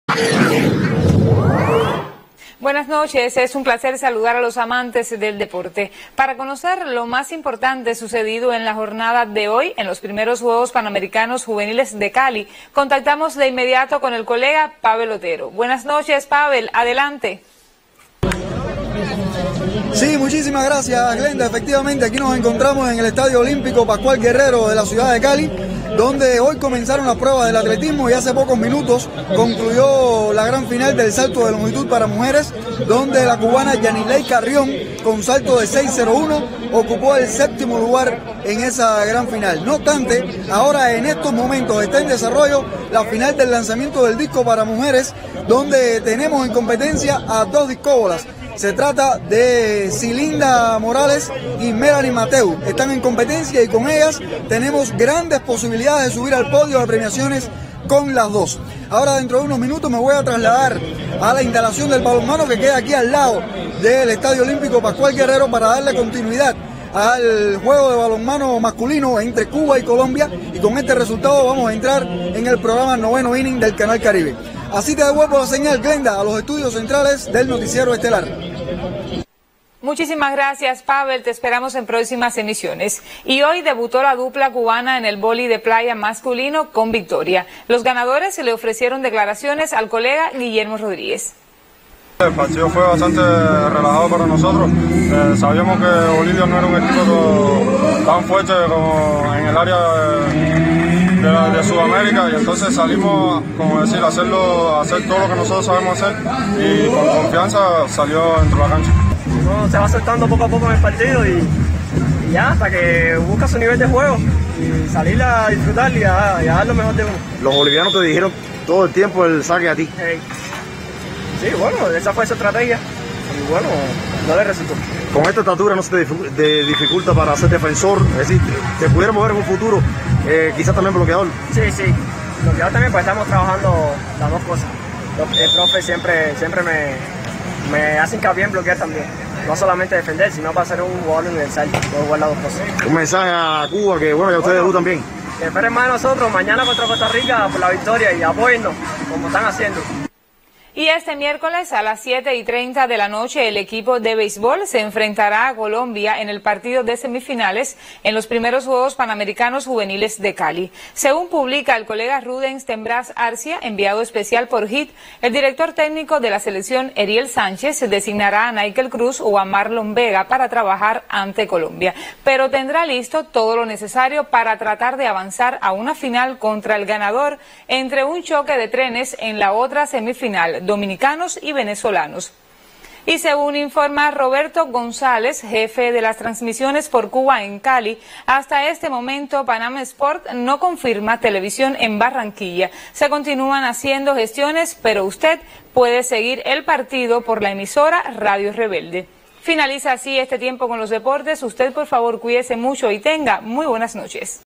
Actualidad Deportiva en Cuba
Todos los detalles importantes de lo sucedido en la jornada de hoy en los primeros JuegosPanamericanos juveniles de Cali.